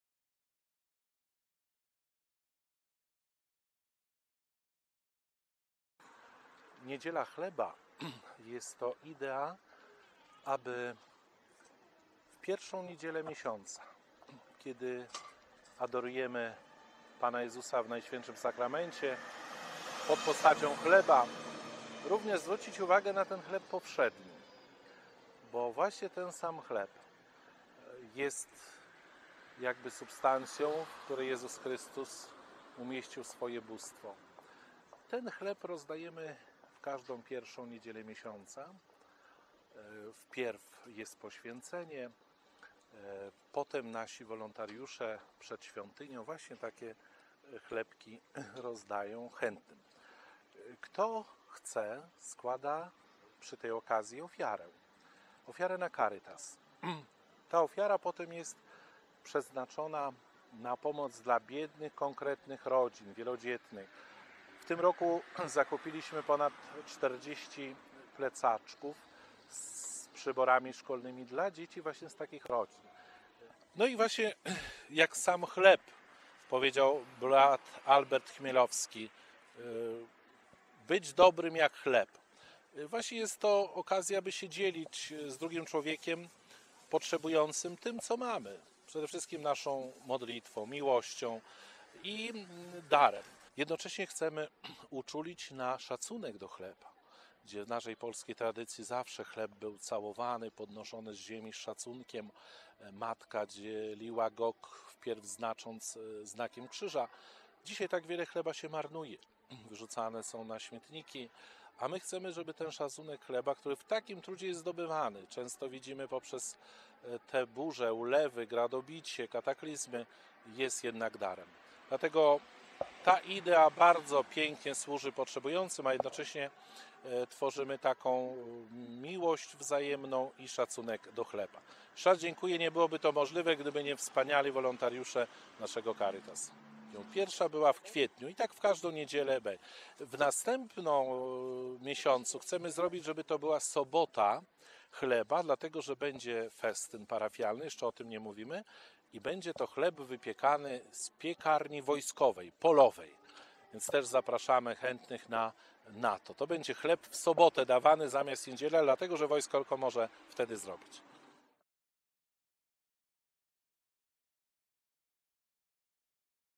Niedziela chleba w legionowskim Kościele Garnizonowym
W parafii pw. Józefa Oblubieńca NMP odbyła się po raz kolejny „Niedziela Chleba". Jest to akcja charytatywna Parafialnego Zespołu Caritas.